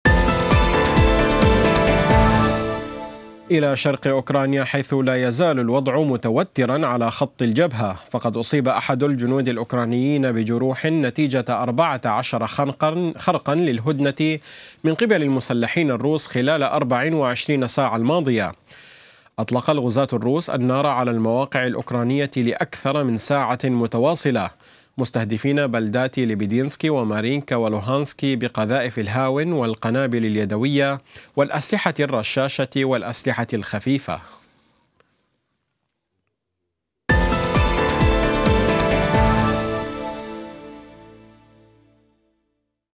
إصابة جندي أوكراني جراء خروقات المسلحين الروس
لا يزال الوضع متوترا على خط الجبهة، فقد أصيب أحد الجنود الأوكرانيين بجروح نتيجة 14 خرقا للهدنة من قبل المسلحين الروس خلال 24 ساعة الماضية.

أطلق الغزاة الروس النار على المواقع الأوكرانية لأكثر من ساعة متواصلة، مستهدفين بلدات ليبيدينسكي ومارينكا ولوهانسك بقذائف الهاون والقنابل اليدوية والأسلحة الرشاشة والأسلحة الخفيفة.